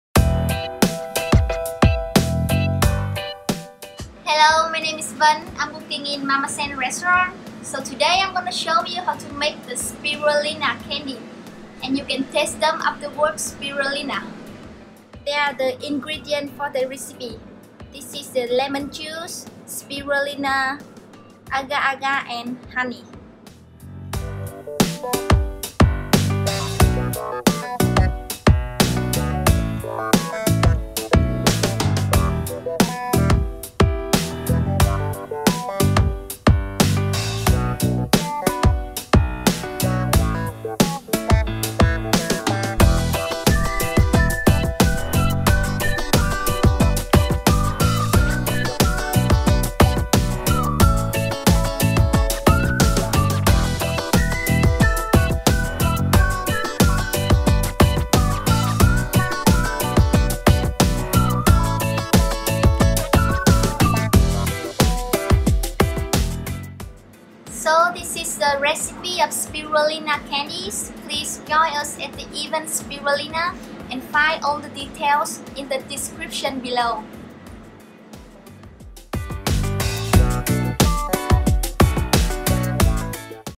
Recipe Spirulina Candies - Bonbons à la spiruline
Les bonbons c'est tellement bon ! 🍡🍡🍡
Vous n'aimez pas le goût de la spiruline sous sa forme naturelle ? Alors, cette recette est pour vous.
"Les Enfants Du Dragon" vous proposent de tester ce produit.
Discover the recipe of spirulina candies, which will be offered during the afterwork spirulina on Thursday, August 1st. Don't resist anymore to curiosity and delicacy, register for the event
Découvrez la recette des bonbons à la spiruline, qui sera offerte lors de la spiruline après-midi le jeudi 1er août. Ne résistez plus à la curiosité et à la gourmandise, inscrivez-vous pour l'événement